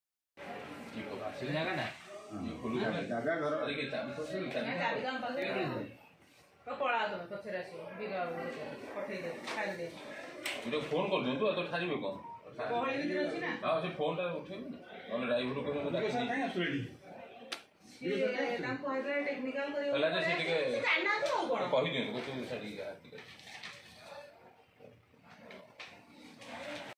OPHWC official throws away bag full of cash
Bhubaneswar: An Odisha Government official attempted to hide a stash of cash by stuffing it into a bag and throwing it into the nearby building as he was raided by Vigilance department sleuths on Tuesday.